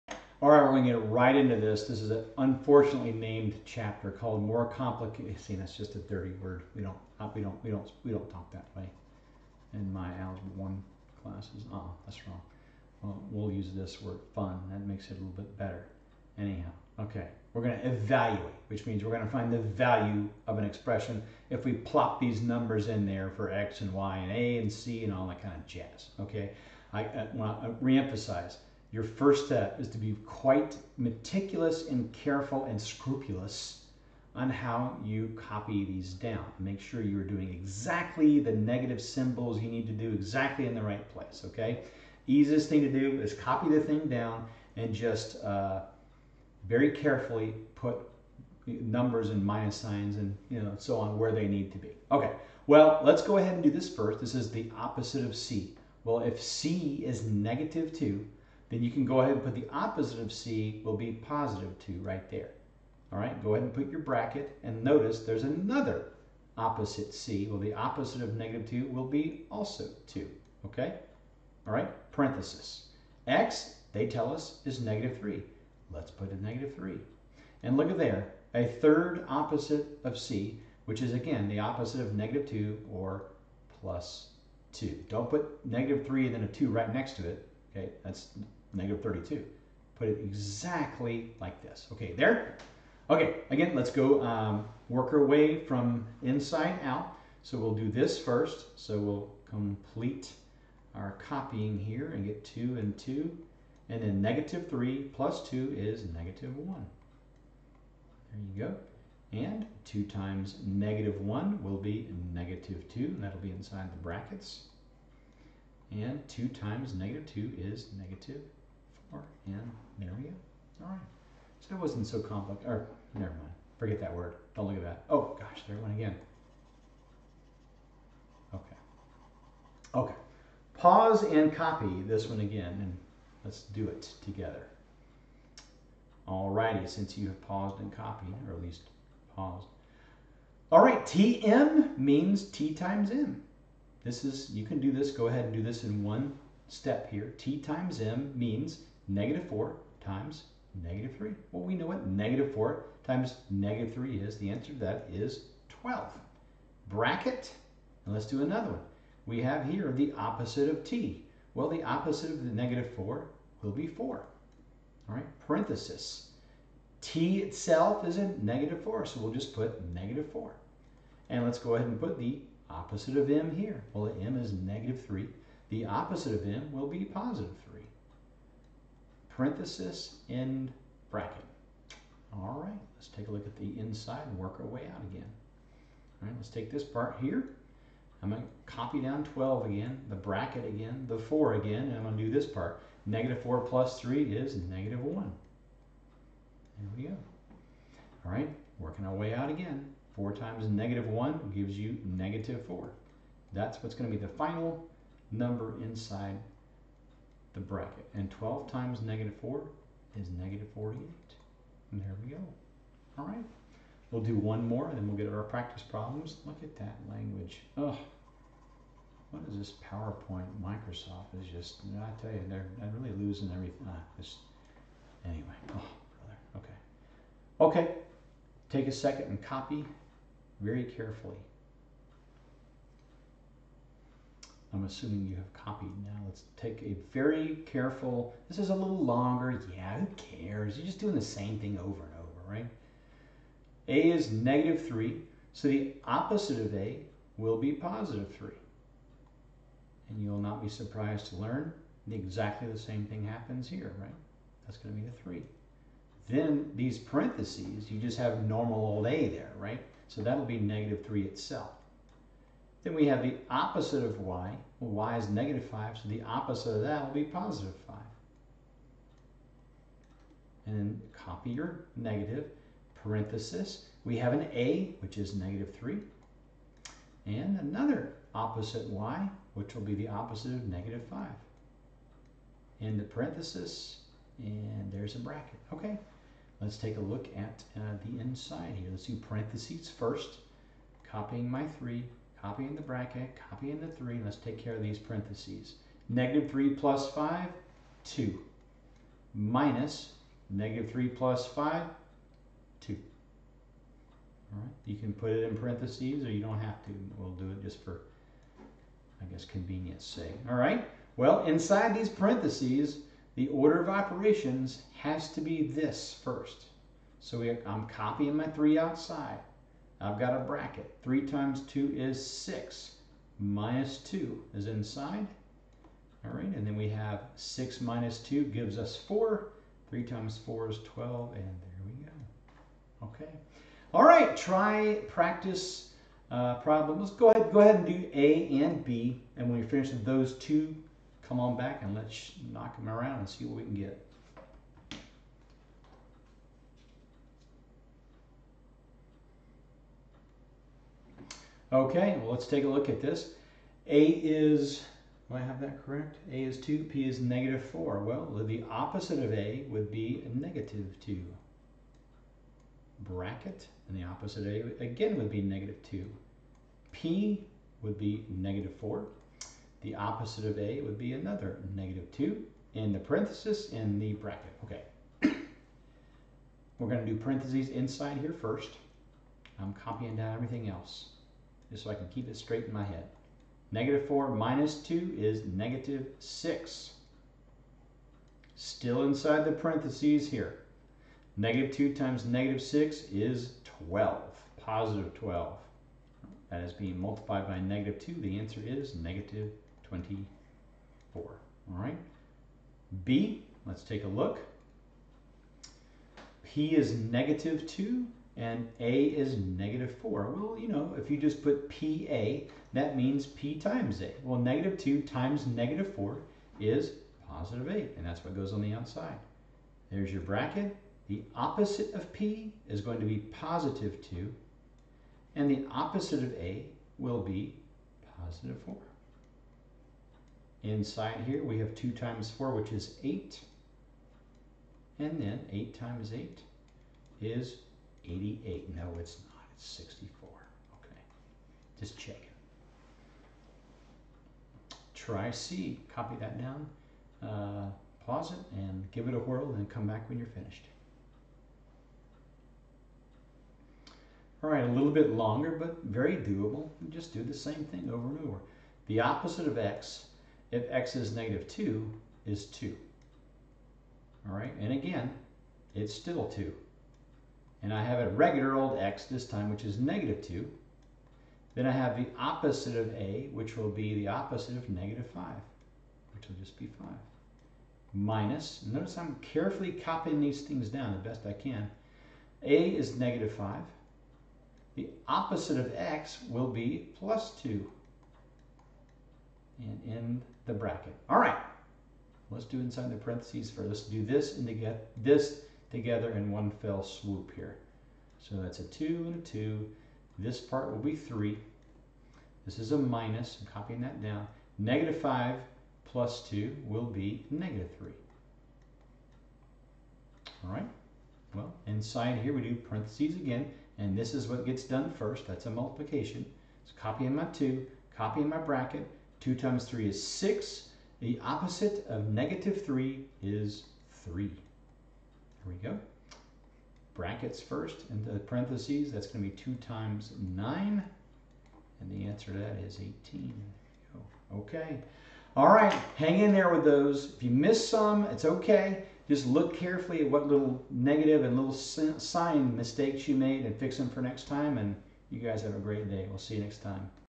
Saxon Math - Algebra 1: 3rd Edition (Lesson 16 - More Complicated Evaluations)
Enjoy this free video lesson for the great John Saxon's Algebra 1 3rd Edition textbook.